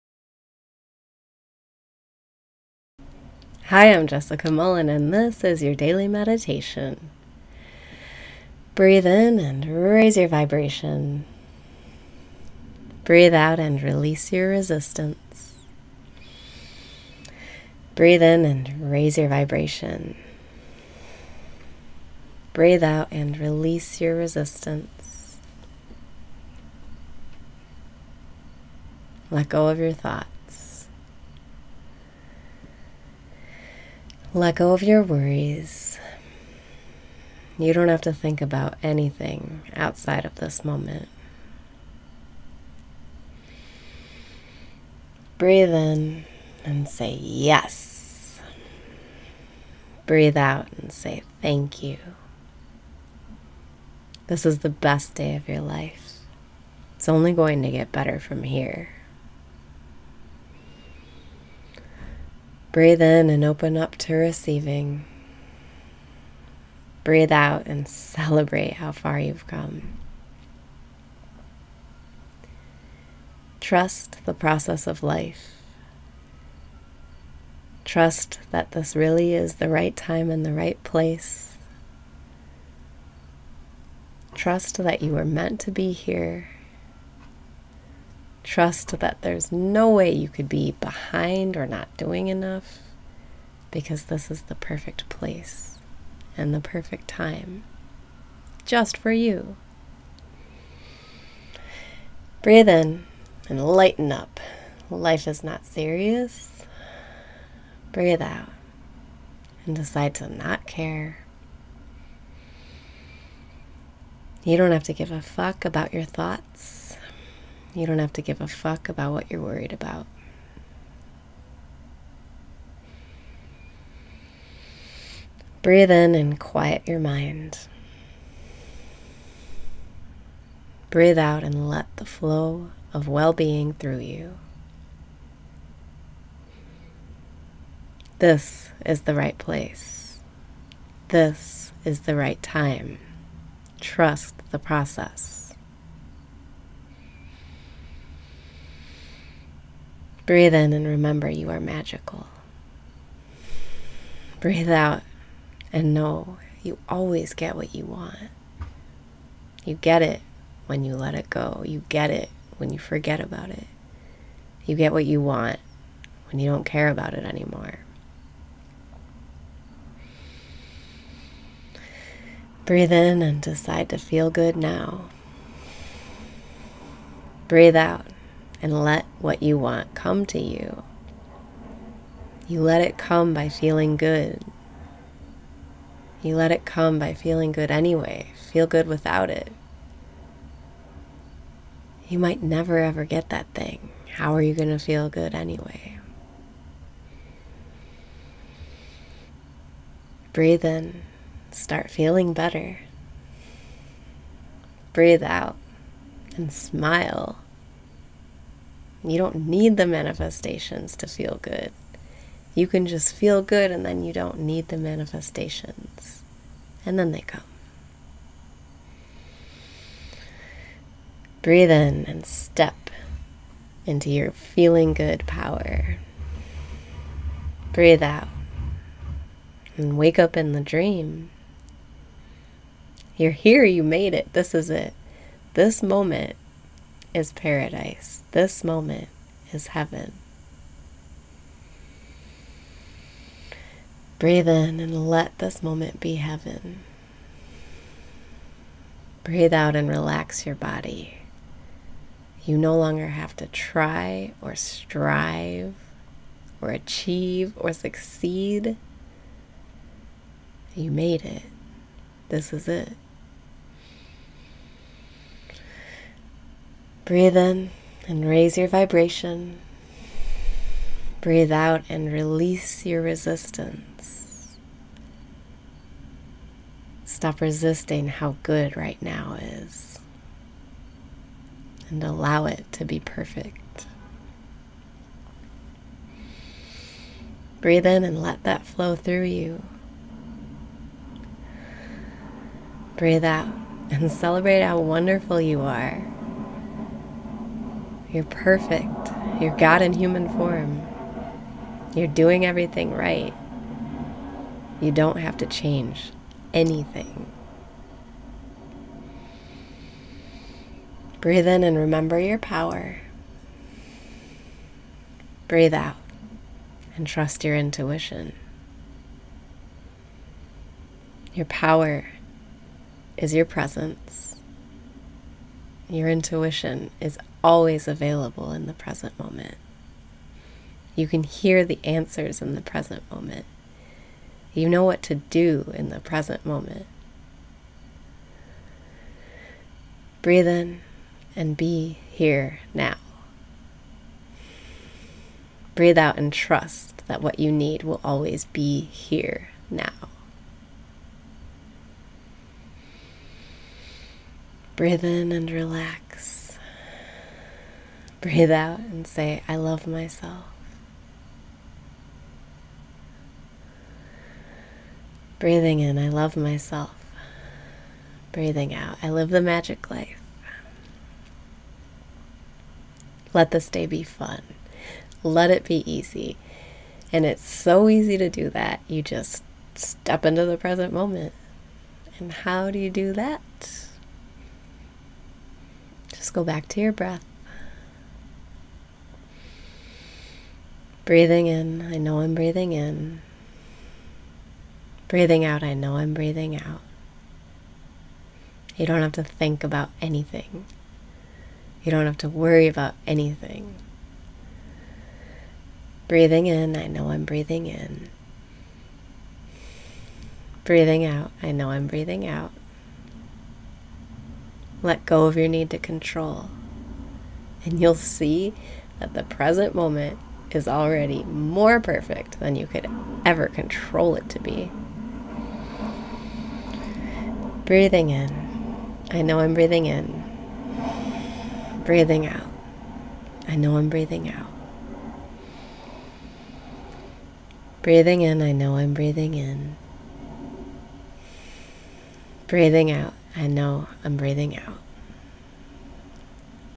Daily Meditation - Your Presence Is Your Power
Let go of your thoughts. Let go of your worries. You don’t have to think about anything outside of this moment.

This is the best day of your life. It’s only going to get better from here.

Trust the process of life. Trust that this really is the right time and the right place. Trust that you were meant to be here. Trust that there’s no way you could be behind or not doing enough, because this is the perfect place and the perfect time, just for you!

Breathe out and decide to not care!

You don’t have to give a fuck about your thoughts. You don’t have to give a fuck about what you’re worried about.

Breathe in and quiet your mind
Breathe out and let the flow of well being through you

This is the right place. This is the right time. Trust the process.

Breathe in and remember you are magickal!
Breathe out and know you always get what you want!

You get it when you let it go. You get it when you forget about it. You get what you want when you don’t care about it anymore.

Breathe in and decide to feel good now!
Breathe out and let what you want come to you!

You let it come by feeling good. You let it come by feeling good anyway. Feel good without it. You might never ever get that thing. How are you going to feel good anyway?

Breathe in and start feeling better!

You don’t need the manifestations to feel good! You can just feel good and then you don’t need the manifestations. And then they come!

Breathe in and step into your feeling-good power!
Breathe out and wake up in the dream!

You’re here! You made it! This is it! This moment is paradise! This moment is heaven!

Breathe in and let this moment be heaven!

You no longer have to try or strive or achieve or succeed! You made it! This is it!

Stop resisting how good right now is. Allow it to be perfect.

Breathe in and let that flow through you!
Breathe out and celebrate how wonderful you are!

You’re perfect! You’re god in human form! You’re doing everything right! You don’t have to change anything!

Breathe in and remember your power!
Breathe out and trust your intuition.

Your power is your presence. Your intuition is always available in the present moment. You can hear the answers in the present moment. You know what to do in the present moment.

Breathe out and trust that what you need will always be here now.

Breathing out I live the magick life!

Let this day be fun! Let it be easy! And it’s so easy to do that, you just step into the present moment! And how do you do that? Just go back to your breath.

You don’t have to think about anything. You don’t have to worry about anything.

Let go of your need to control and you’ll see that the present moment is already more perfect than you could ever control it to be!